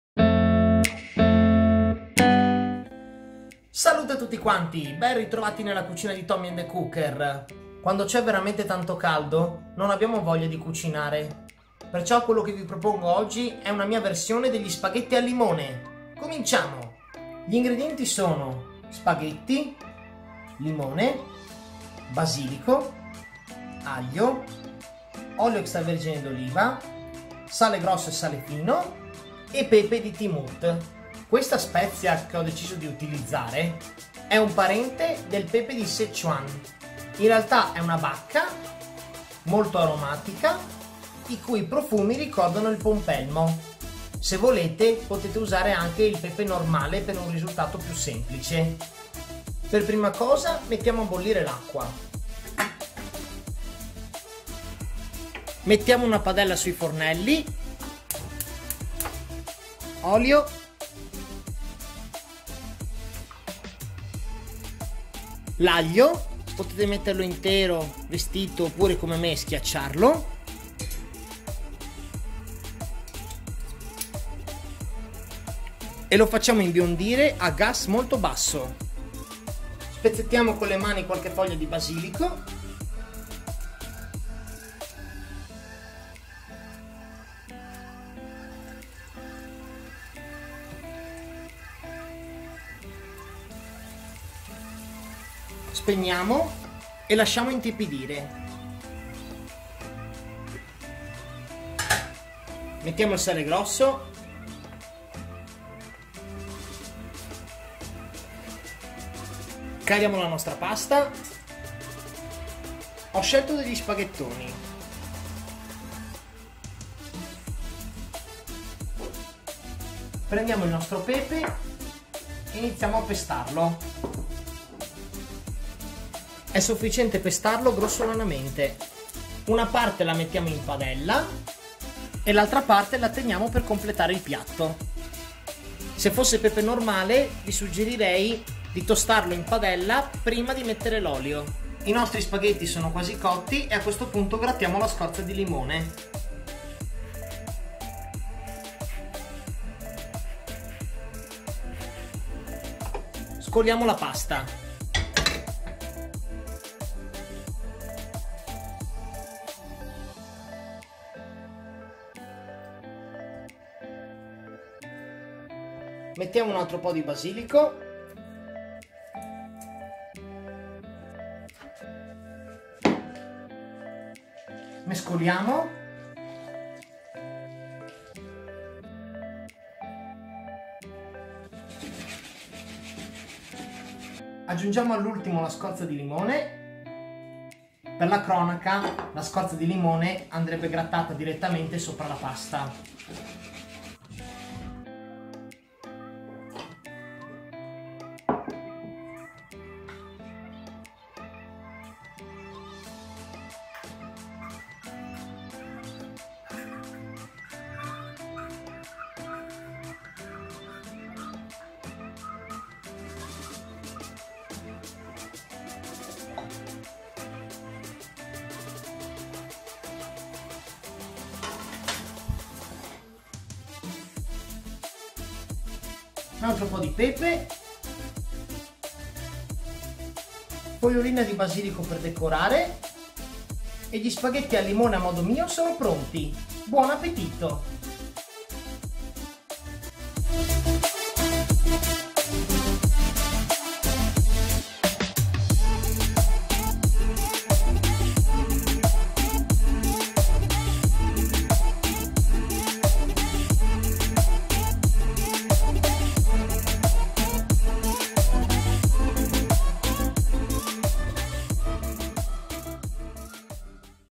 Spaghetti al limone
Estate, caldo, fame di qualcosa di fresco e veloce perché solo a guardare i fornelli...
Allora uno spaghettino veloce, veloce e senza troppe pretese, ma originale e versatile.
Vi propongo oggi gli spaghetti al limone come li facciamo spesso noi a casa, una sorta di piatto della protesta. Mette tutti d'accordo.
Buon appetito!